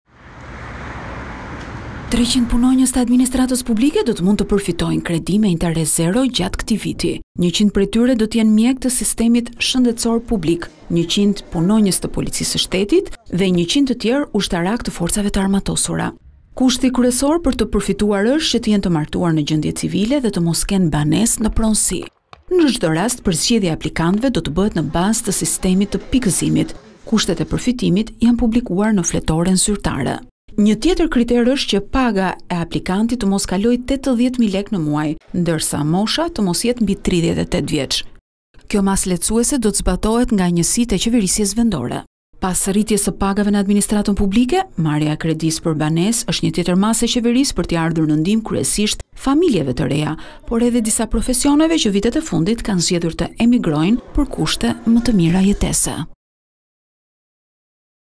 300 kredi te buta per administraten. Perfitojne mjeket, policet dhe ushtaraket -
300 punonjës të administatës publike do të mund të përfitojnë kredi me interes zero, gjatë këtij viti. 100 prej tyre do të jenë mjekë të sistemit shëndetësor publik, 100 punonjës të policisë së shtetit dhe 100 të tjerë ushtarakë të forcave të armatosura. Kushti kryesor për të përfituar është që të jenë të martuar në gjëndje civile dhe të mos kenë banesë në pronësi.  Në cdo rast përzgjedhja e aplikantëve do të bëhet në bazë të sistemit të pikëzimit. Kushtet e përfitimit janë publikuar në fletoren zyrtare. Një nga kriteretet është që paga e tyre të mos kalojë 80 mijë lekë në muaj, ndërsa mosha e aplikantit të mos jetë mbi 38 vjec.  Pas rritjes së pagave në administratën publike, marrja e kredisë për banesë është një tjetër masë e qeverisë për ti ardhur në ndihmë kryesisht familjeve të reja por edhe disa profesioneve që vitet e fundit kanë zgjedhur të emigrojnë për kushte më të mira jetese.